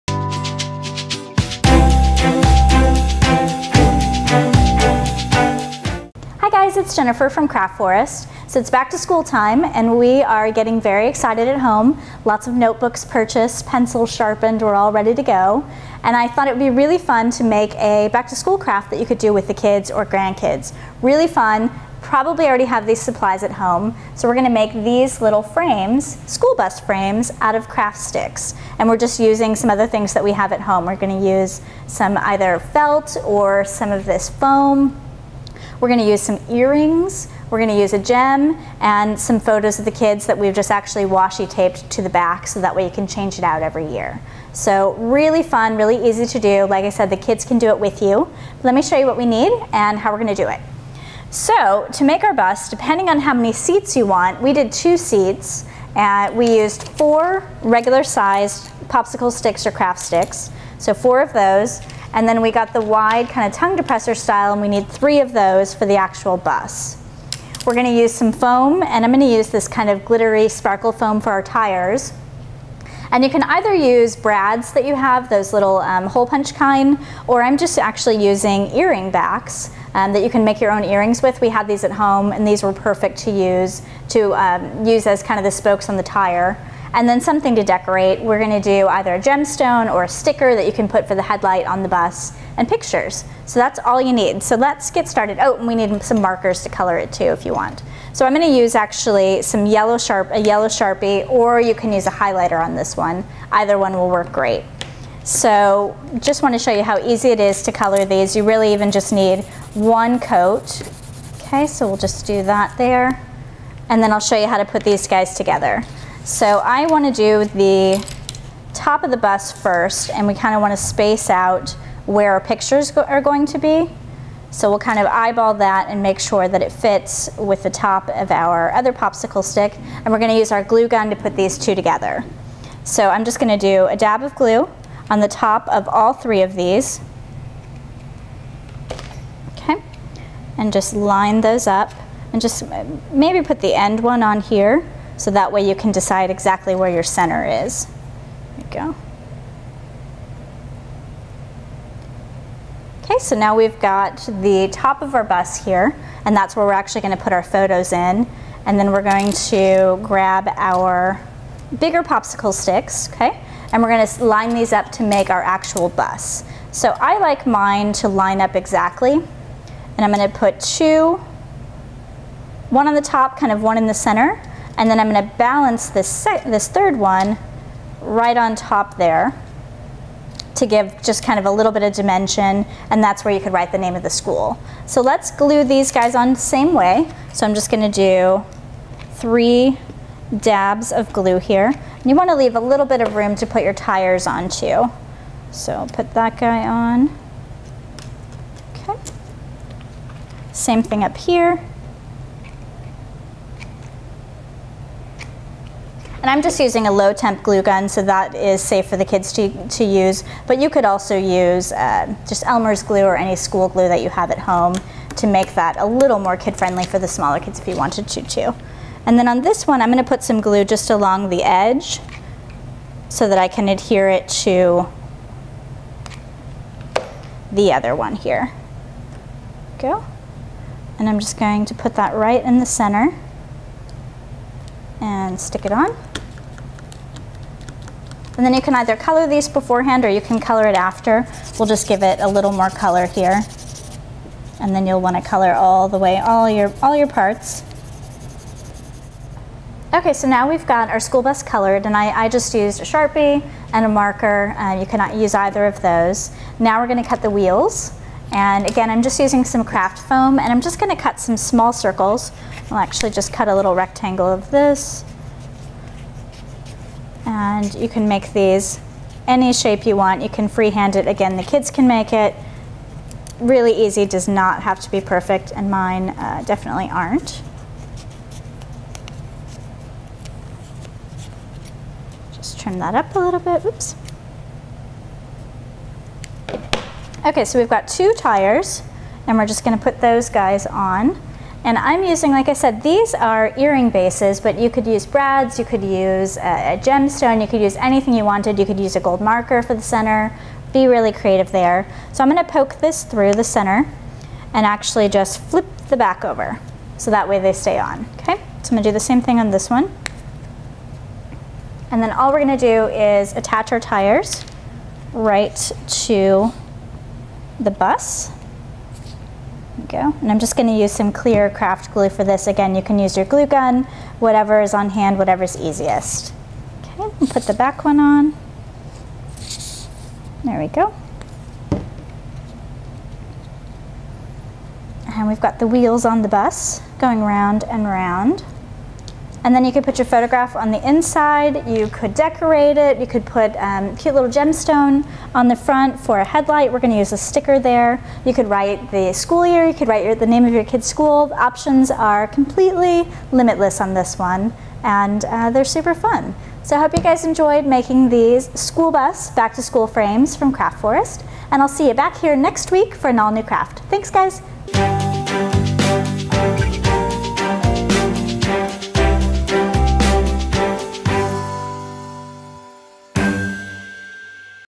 Back to School Craft for Kids: School Bus Picture Frames
It's back to school time! Make your own school bus picture frames from popsicle sticks! This is the perfect back to school craft for the kids. And when you're done they make great gifts for grandparents and teachers!